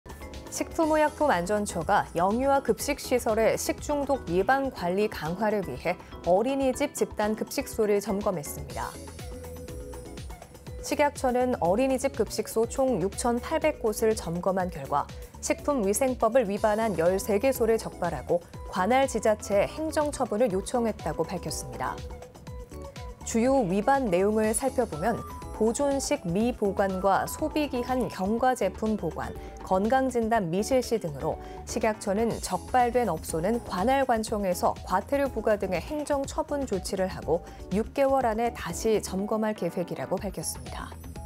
[소셜] 사회ㅣ어린이집 급식소 점검, ‘식품위생법’ 위반 13개소 적발 (이한승)ㅣCTS뉴스
식품의약품안전처가 영유아 급식시설의 식중독 예방관리 강화를 위해 어린이집 집단 급식소를 점검했습니다.

식약처는 어린이집 급식소 총 6,800곳을 점검한 결과 ‘식품위생법’을 위반한 13개소를 적발하고 관할 지자체에 행정 처분을 요청했다고 밝혔습니다.

주요 위반 내용으로는 보존식 미보관과 소비기한 경과 제품 보관, 건강진단 미실시 등으로 식약처는 적발된 업소는 관할 관청에서 과태료 부과 등 행정처분 조치하고 6개월 안에 다시 점검할 계획입니다.